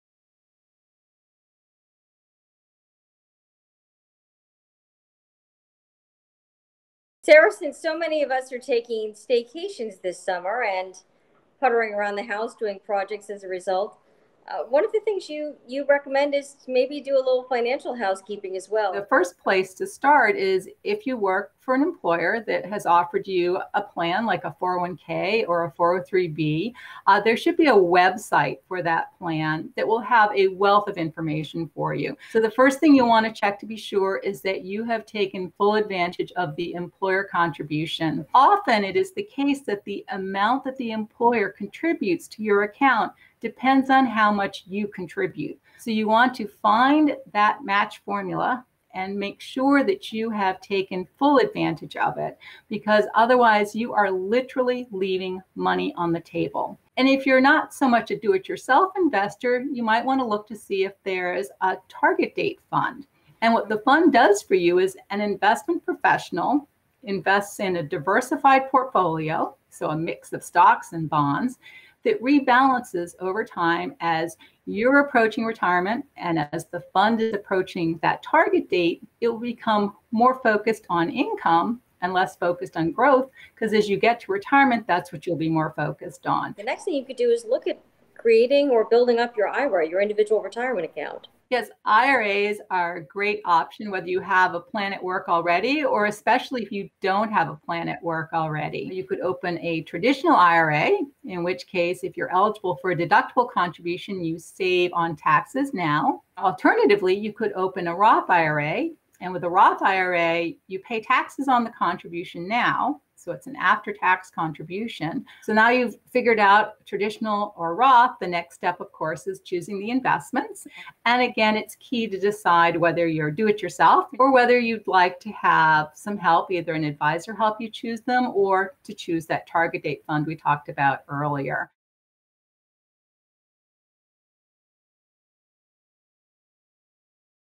An End-of-Summer Checkup for Your Long-Term Savings
Now is the perfect time to ensure your long-term savings are on track. In the August 28, 2020, edition of Focus on Funds, ICI Senior Director of Retirement and Investor Research Sarah Holden offers a helpful checklist.

Stephanie Ortbals-Tibbs, ICI director of media relations: Sarah, since so many of us are taking staycations this summer and puttering around the house doing projects as a result, what are the things you recommend us to maybe do a little financial housekeeping as well?

Sarah Holden, ICI senior director of retirement and investor research: The first place to start is if you work for an employer that has offered you a plan like a 401(k) or a 403(b), there should be a website for that plan that will have a wealth of information for you.

So the first thing that you want to check to be sure is that you have taken full advantage of the employer contribution. Often, it is the case that the amount that the employer contributes to your account depends on how much you contribute. So you want to find that match formula and make sure that you have taken full advantage of it—because otherwise, you are literally leaving money on the table.

And if you’re not so much a do-it-yourself investor, you might want to look to see if there is a target date fund. And what the fund does for you is an investment professional invests in a diversified portfolio—so a mix of stocks and bonds—that rebalances over time as you’re approaching retirement. And as the fund is approaching the target date, it’ll become more focused on income and less focused on growth, because as you get to retirement, that’s what you’ll be more focused on.

Ortbals-Tibbs: The next thing you could do is look at creating or building up your IRA—your individual retirement account.

Holden: Yes, IRAs are a great option whether you have a plan at work already, or especially if you don’t have a plan at work already.

You could open a traditional IRA—in which case, if you’re eligible for a deductible contribution, you save on taxes now. Alternatively, you could open a Roth IRA. And with a Roth IRA, you pay taxes on the contribution now—so it’s an after-tax contribution.

So now you’ve figured out traditional or Roth—the next step of course, is choosing the investments. And again, it’s key to decide whether you’re do-it-yourself, or whether you’d to have some help—either an adviser help you choose them or to choose that target date fund we talked about earlier.